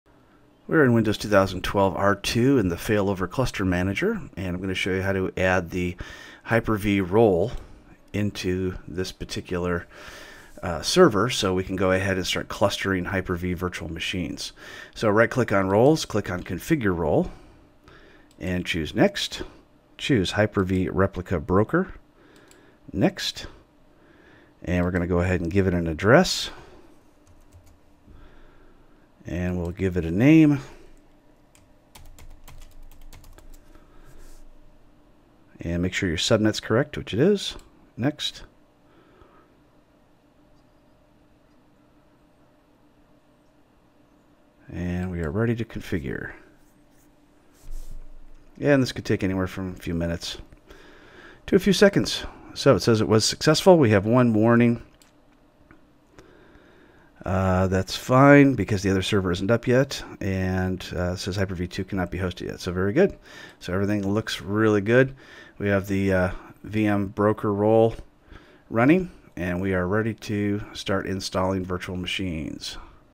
How to install the Hyper V Replica Broker Role in Cluster Manager 2012 R2
Author, teacher, and talk show host Robert McMillen shows you how to install the Hyper V Replica Broker Role in Cluster Manager 2012 R2